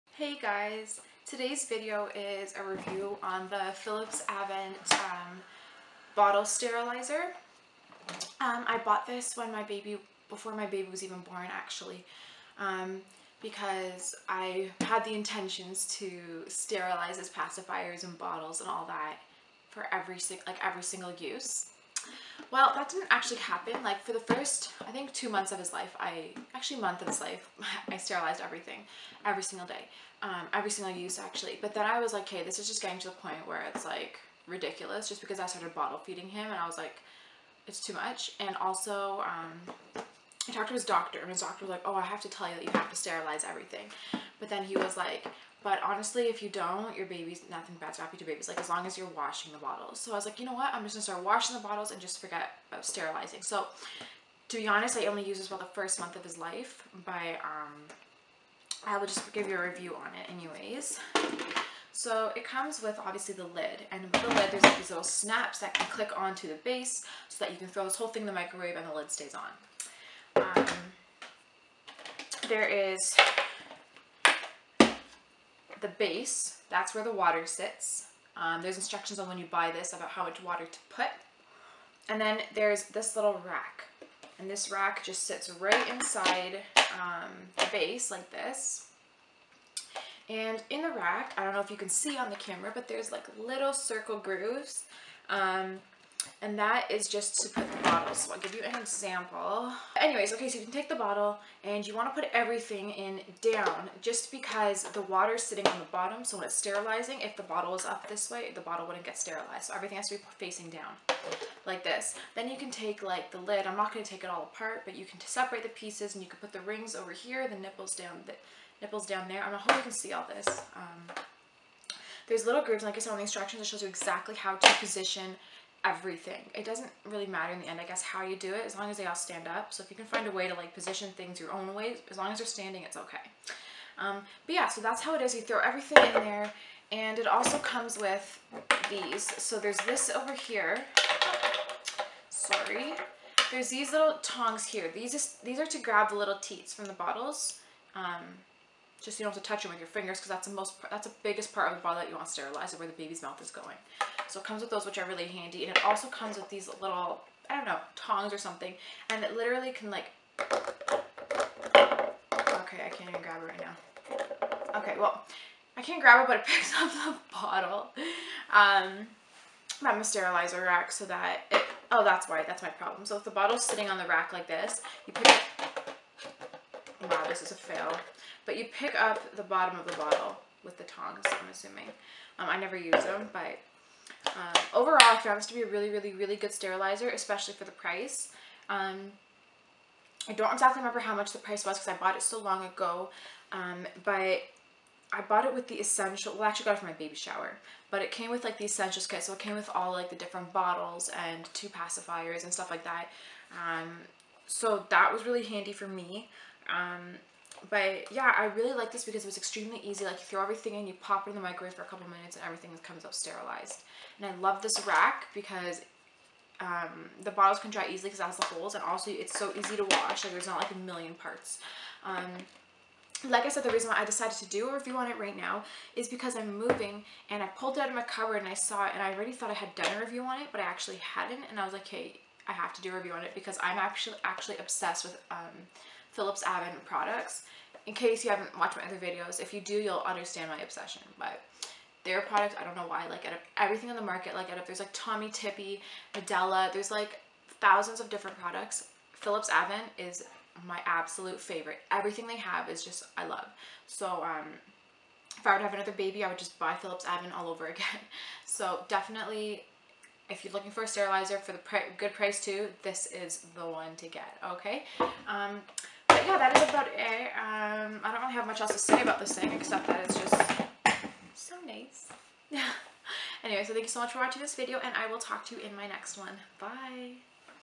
Philips AVENT Microwave Steam Sterilizer Review
Here is a Review on the Philips AVENT microwave steam sterilizer! If you are a looking for a good, cheap bottle steam sterilizer than this one is the best there is!